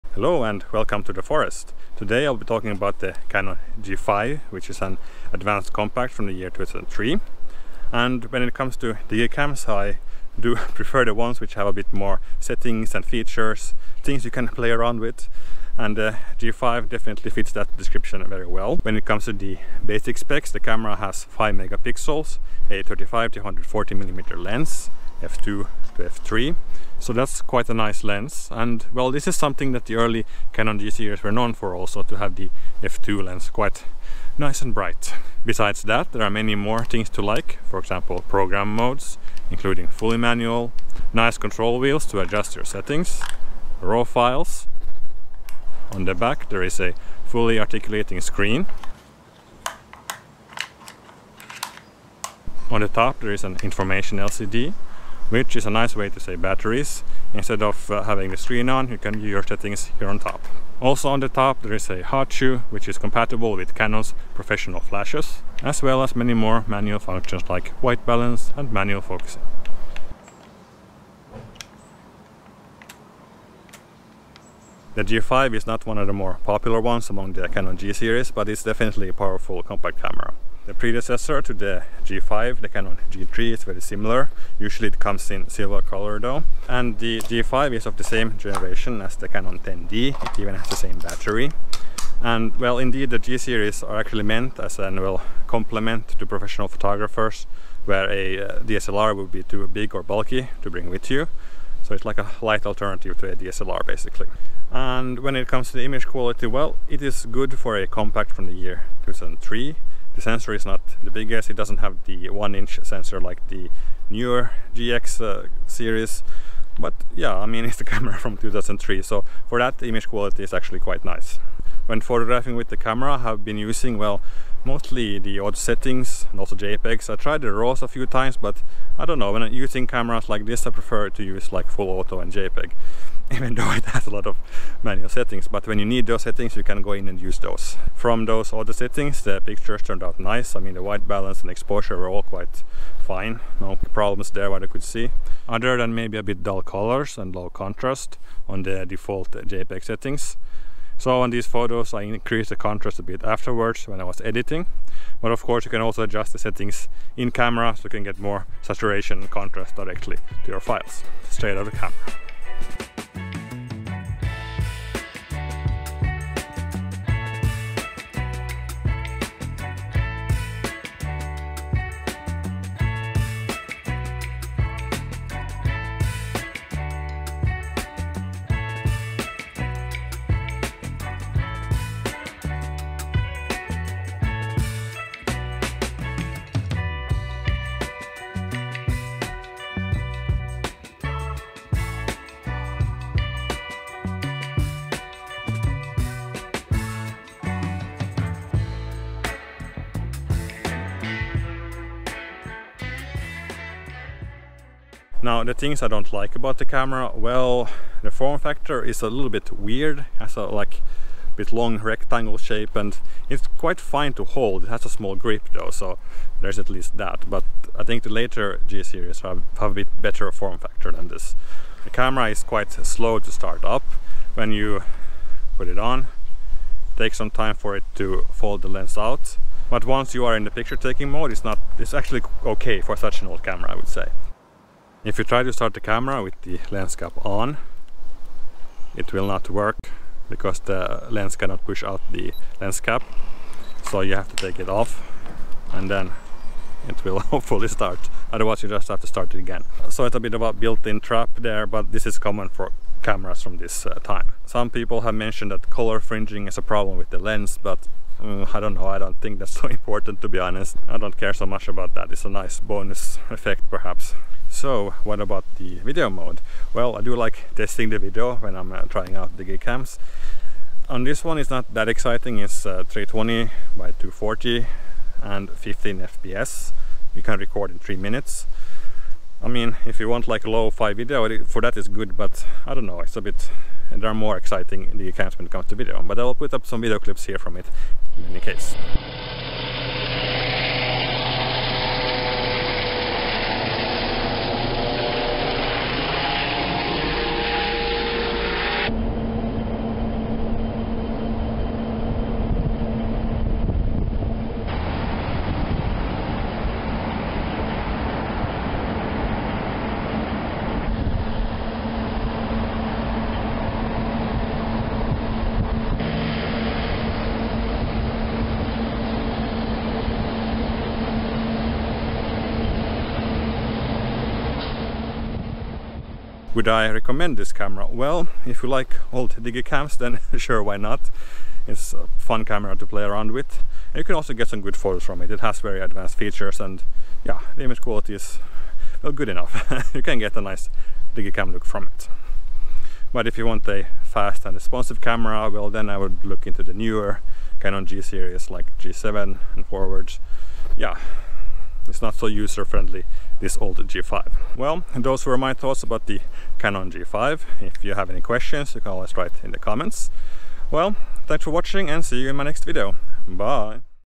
Canon G5 - Advanced Digicam from 2003 with a Great Lens!
The Canon Powershot G5 is an advanced digicam from 2003. Not the most popular in the Canon G-series and maybe not so compact compared to later ones. But it is definitely filled with advanced features and produces quite nice photos!

Chapters:
0:00 Intro / Main Specs
1:44 Image Quality & Photos
3:25 Bad Things
4:24 Video & Samples
5:26 Recommendations